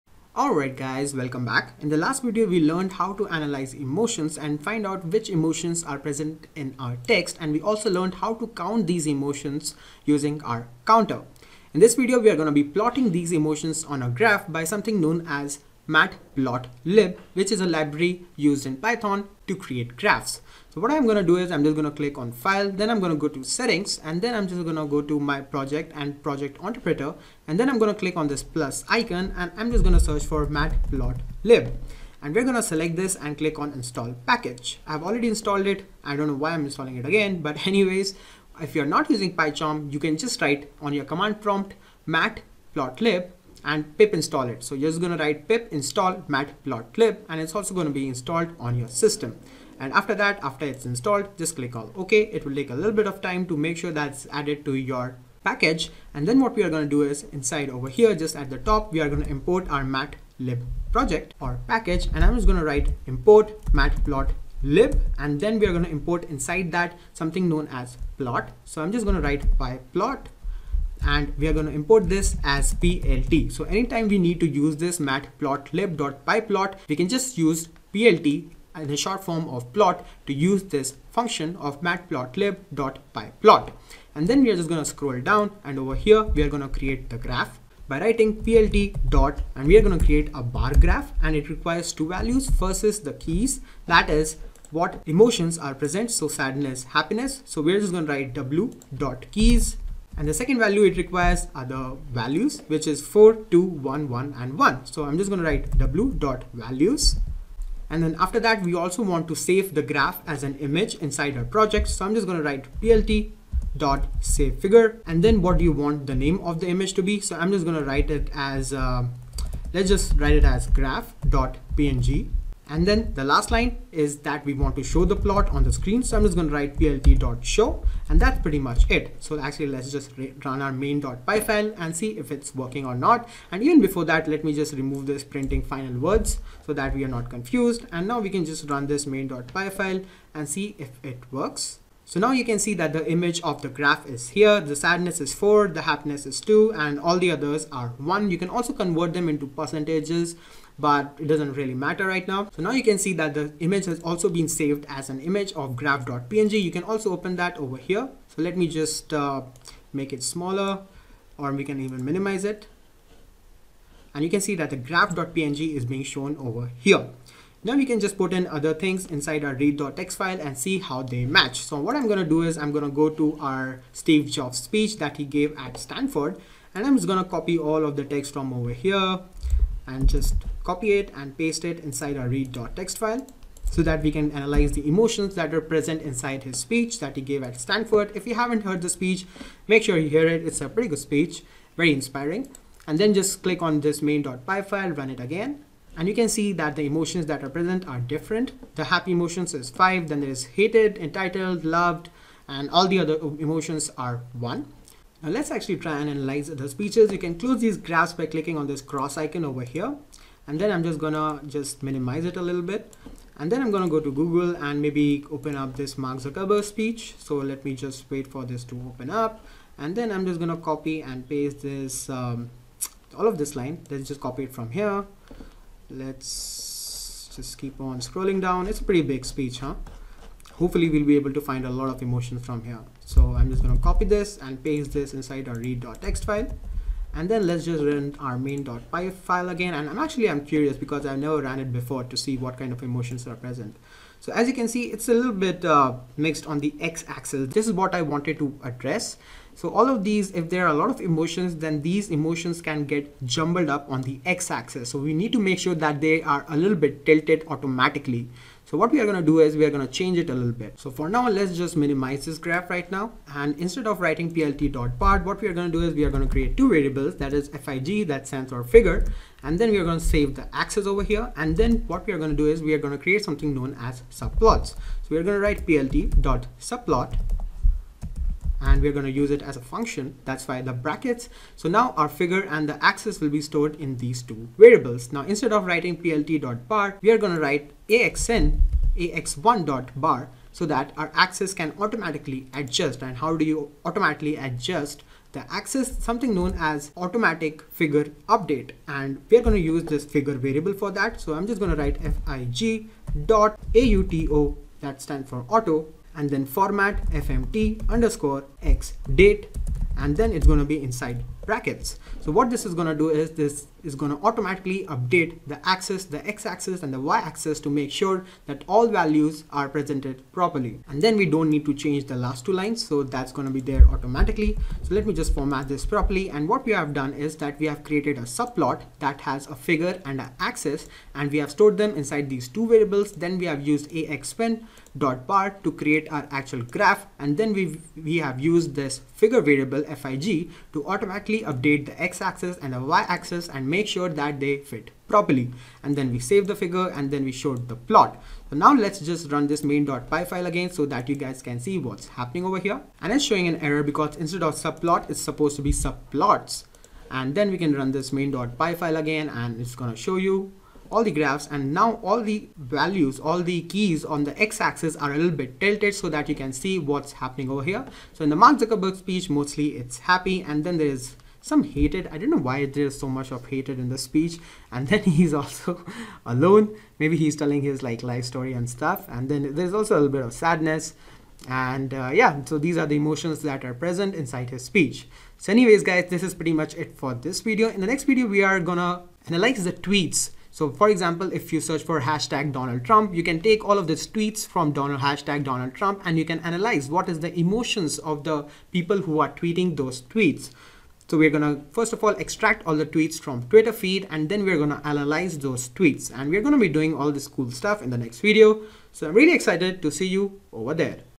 Sentiment Analysis Python - 7 - Emotions in a Graph using Matplotlib (NLP)
In this video, we will be displaying emotions in a bar graph using Matplotlib.